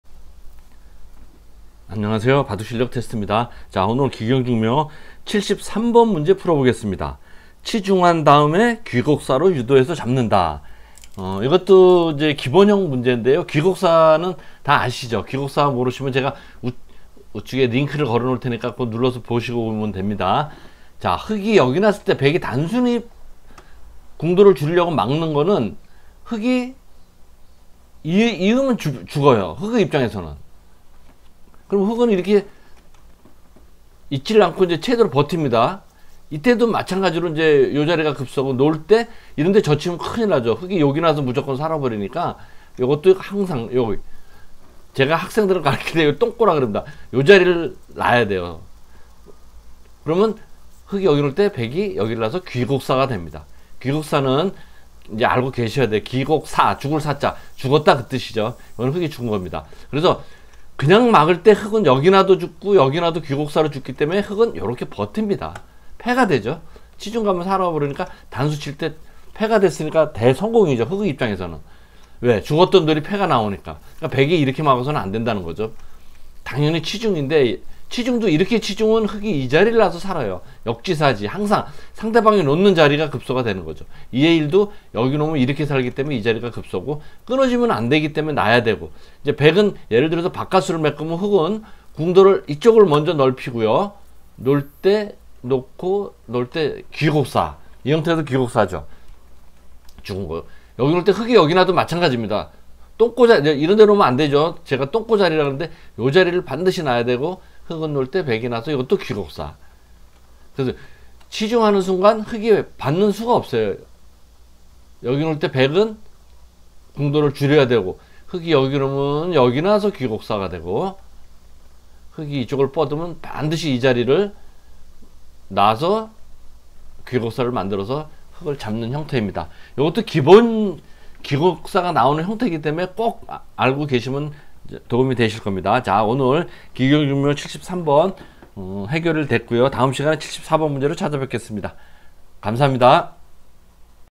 碁經衆妙[기경중묘] - 73(백선)
碁經衆妙[기경중묘] - 73
바둑의 기술이란 것이 따지고 보면 언제나 돌의 사활로 귀결된다.
세계적 고수들이 틈날 때마다 사활 문제에 매달리는 것은 감각 단련과 기본기 단련에 최상의 방법이기 때문이다.
 “나는 13세 때 우연히 玄玄棋經[현현기경]을 입수한 뒤 무아경 속에서 문제풀이에 열중했다.
고작 6급 실력이었던 나는 그 후부터 사활에 엄청난 자신감이 생겨 고수들을 두려워하지 않게 됐다.” ‘영원한 국수’ 김인 9단의 회상이다.

인생의 묘수가 필요한 때, 바둑 6대 고전을 펴라!
圍棋古典叢書[위기고전총서]
1 : 玄玄棋經[현현기경]
2 : 官子譜(上)[관자보상]
3 : 官子譜(下)[관자보하]
4 : 發陽論. 玄覽[발양론.현람]
5 : 碁經衆妙[기경중묘]
6 : 死活妙機[사활묘기]